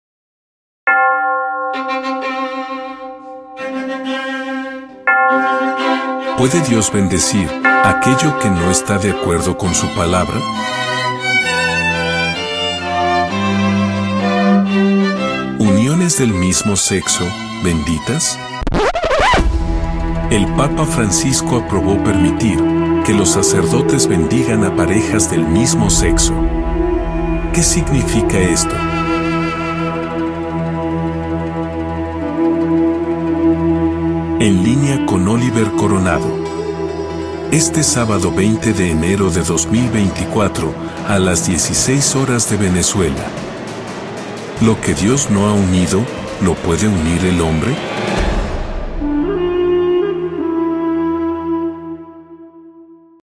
UNIONES DEL MISMO SEXO ¿BENDITAS? | PROMO
UNIONES DEL. MISMO SEXO ¿BENDITAS?

Lo que Dios no ha unido ¿Lo puede unir el hombre?

Levítico 20:13 "Si un hombre se acuesta con varón como se acuesta con mujer, cometen una abominación."

📌 FECHA: Hoy Sábado 20 de Enero de 2024.

HORA: 16 horas de Venezuela.

15:00 Horas 🇵🇪 🇪🇨 🇵🇦 🇨🇴 🇲🇽 🇨🇴
16:00 Horas 🇻🇪 🇺🇸🇧🇴 🇧🇷 🇵🇷 🇩🇴 🇨🇺
17:00 Horas 🇦🇷 🇵🇾 🇨🇱
21:00 Horas 🇪🇸

📌 CORONA STUDIO TEAM

y recibe todo este hermoso material.

COMPARTE Y ESTUDIA CON NOSOTROS

CORONA STUDIO Team